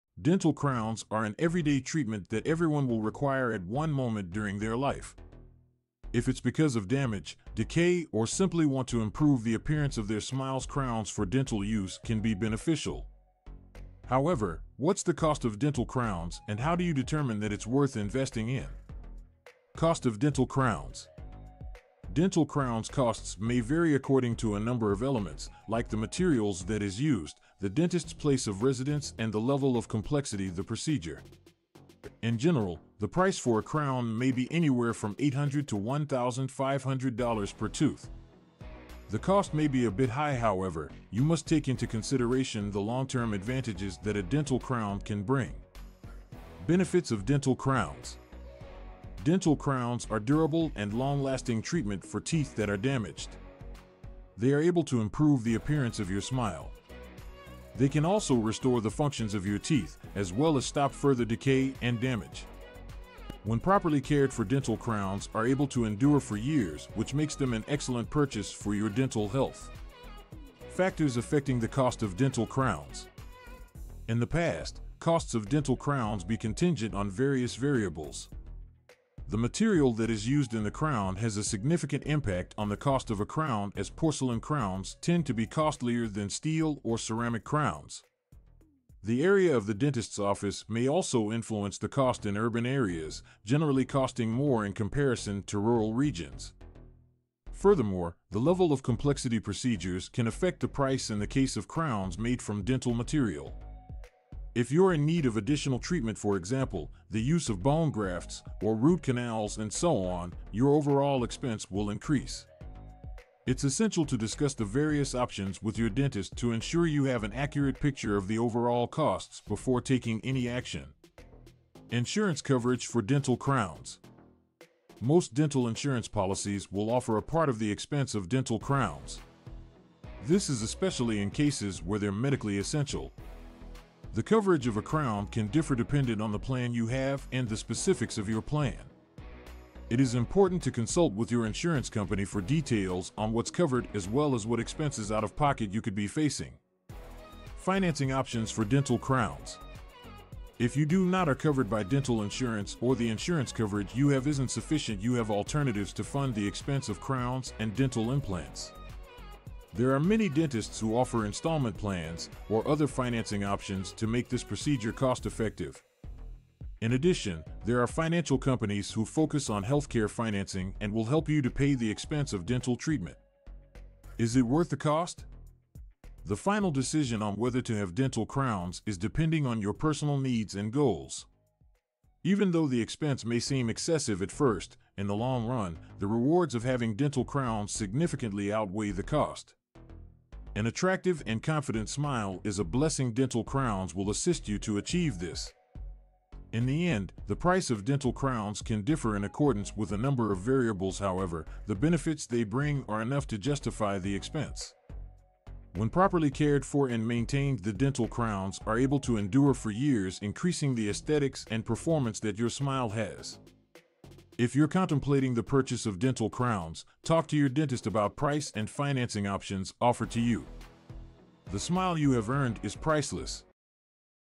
Transform Your Smile with Dental Crowns - Is This the Solution to Your Dental Problems?
Dental crowns are an everyday treatment that everyone will require at one moment during their life. If it's because of damage, decay or simply want to improve the appearance of their smiles Crowns for dental use can be beneficial. However, what's the cost of dental crowns and how do you determine that it's worth investing in? dental crowns, treatment, damage, decay, appearance, smile, everyday, improve, beneficial